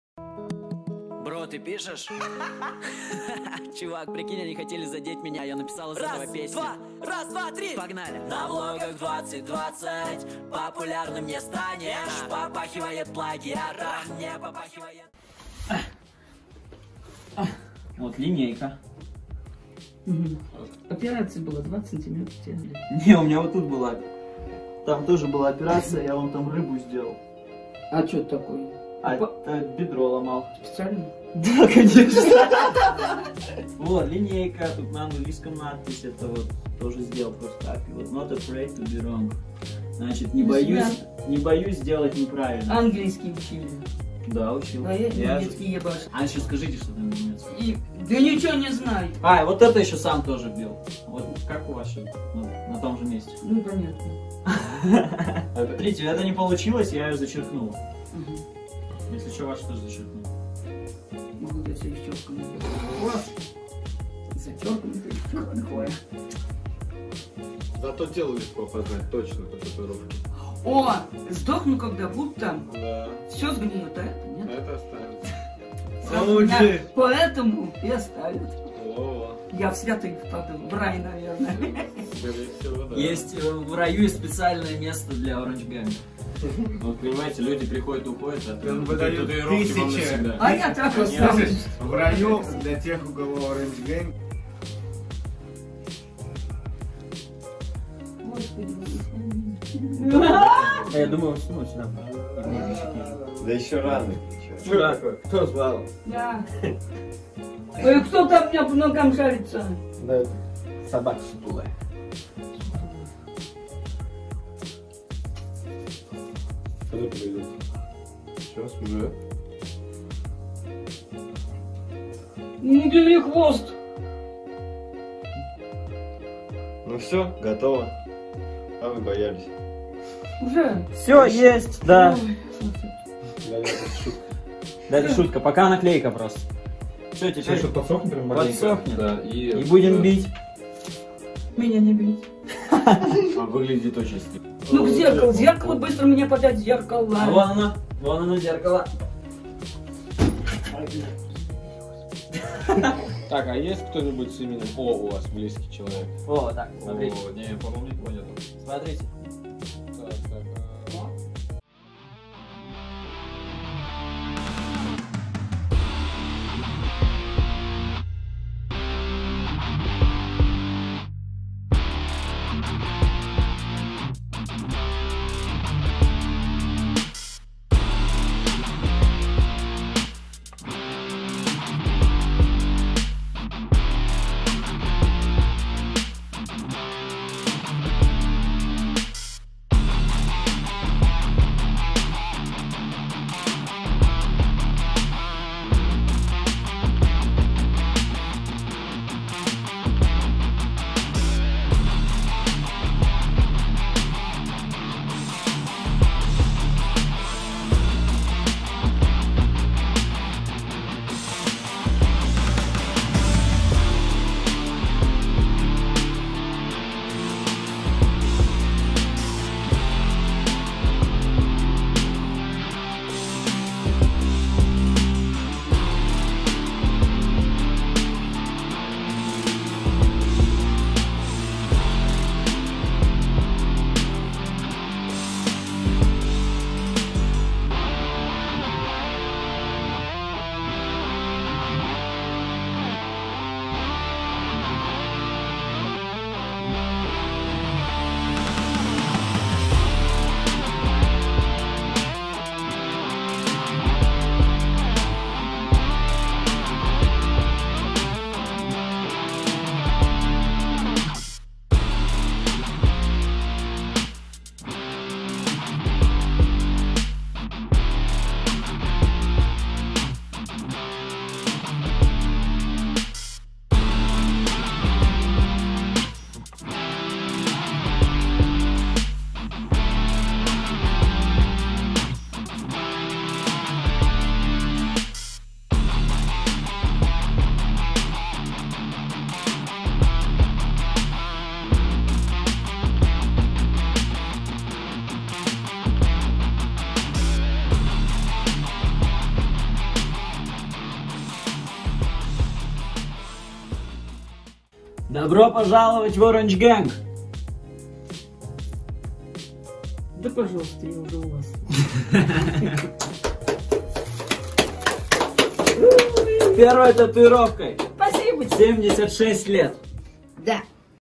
ПЕРВОЕ ТАТУ В 76 ЛЕТ БАБУШКЕ ХЕЙТЕРУ!
ПЕРВОЕ ТАТУ В 76 ЛЕТ БАБУШКЕ ХЕЙТЕРУ! ОТ СЭМА ПОЛЫВЯННОГО. СЭМ РАССКАЗЫВАЕТ ПРО СВОИ ТАТУИРОВКИ. ДОБРО ПОЖАЛОВАТЬ В OG(ORANGEGANG)!!!